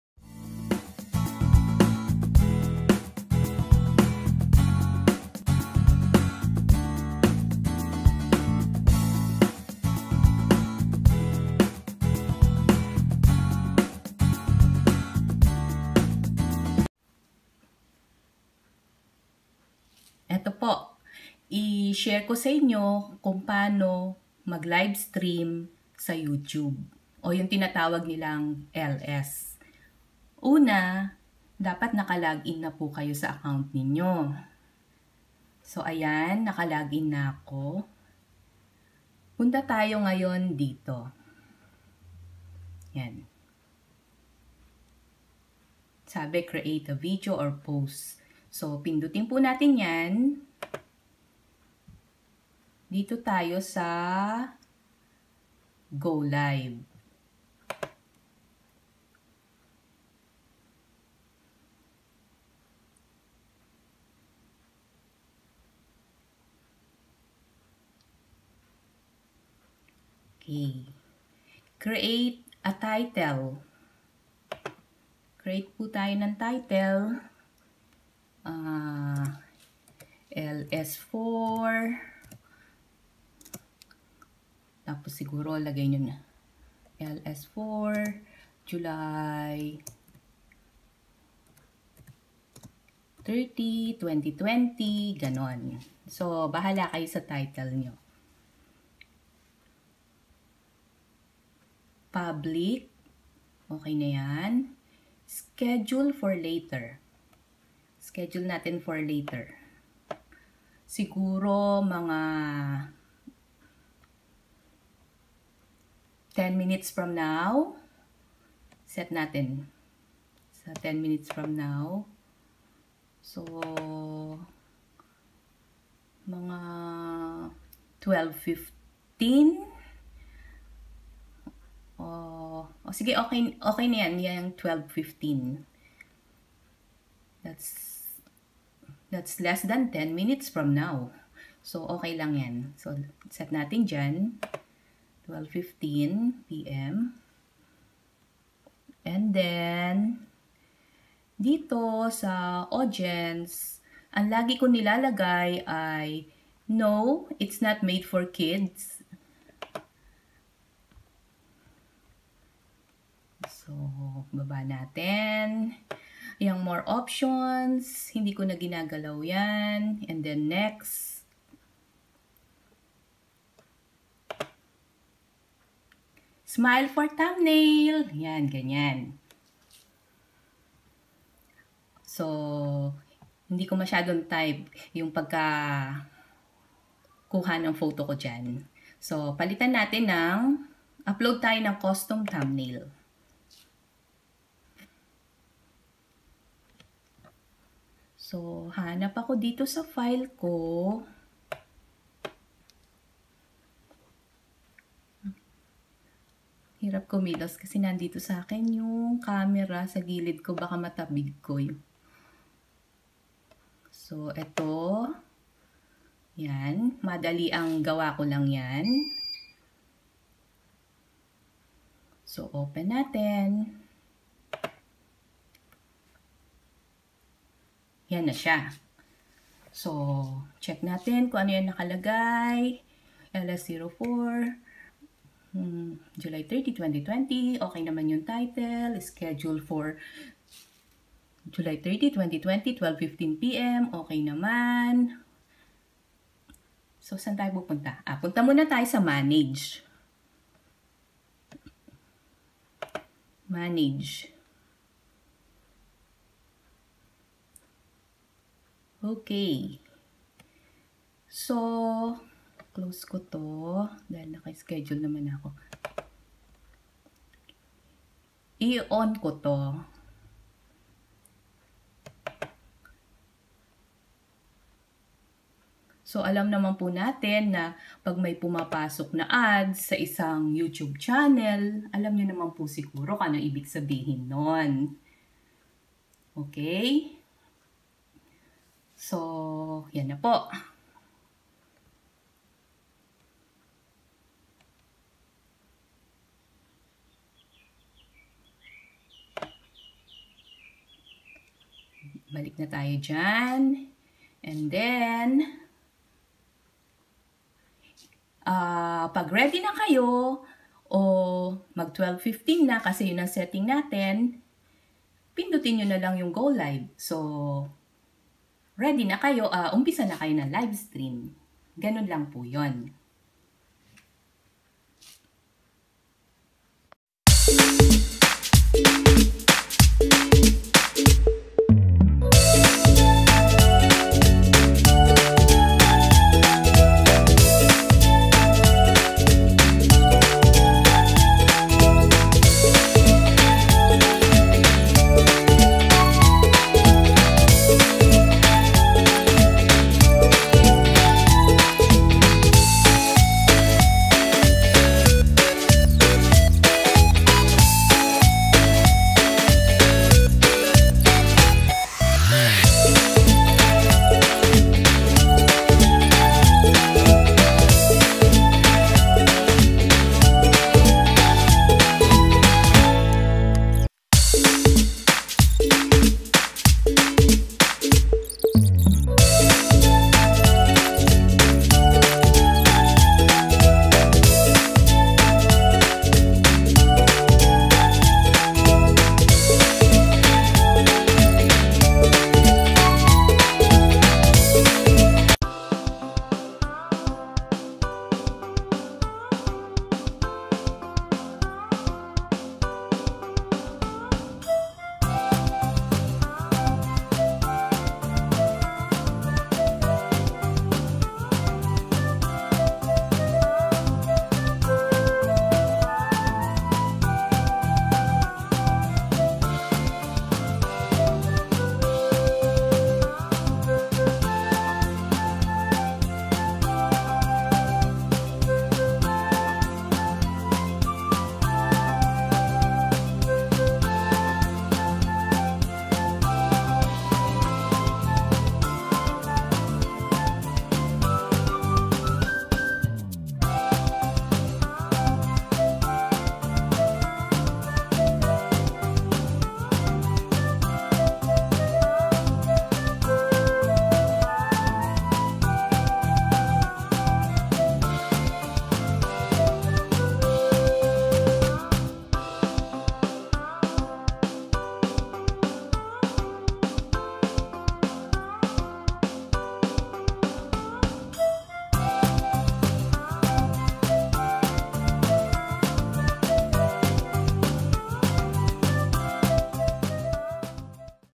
Paano mag livestream gamit ang laptop? (Direct sa YT channel mo)
Tutorial po para sa mga gustong mag LS (Isa sa mga paraan ng pag LS gamit ang laptop)